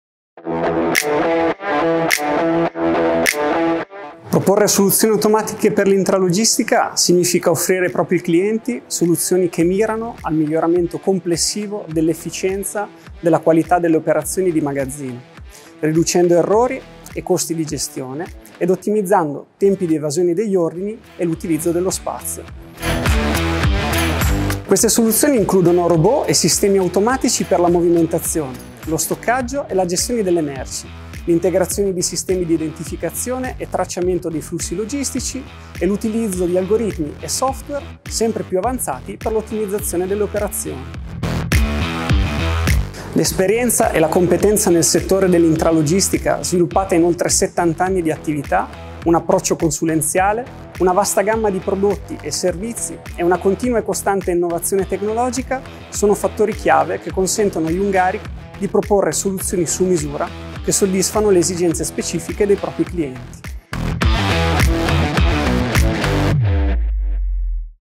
L’automazione secondo Jungheinrich - Il magazzino automatico su misura
Siamo profondamente convinti che non possa esistere un porgetto di automazione uguale all’altro. Per questo proponiamo solo soluzioni su misura che soddisfino le esigenze specifiche dei nostri clienti. Per farlo, ci avvaliamo di un approccio consulenziale, una vasta gamma di prodotti e servizi e una continua e costante innovazione tecnologica.